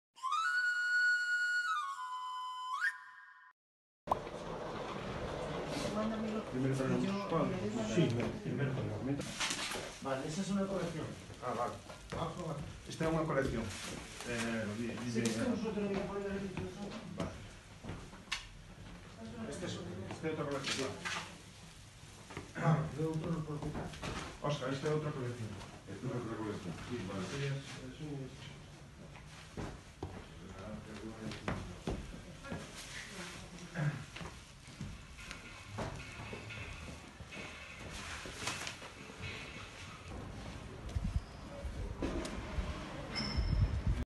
Traslado de los cuadros de Julio Rivera
Ayer por la mañana se trasladaron las pinturas de Julio Rivera desde la tienda de su hermano Luis, en Doutor Marañón, para trasladarlas a la Sala Valente. En este espacio se inaugurará el jueves 7 de julio, a las 20,30 horas, una gran exposición con 70 originales del artista ourensano. La muestra está comisariada por Eva Torres y la organiza Asociación Cultural Memorial Julio Rivera, presidida por Luis.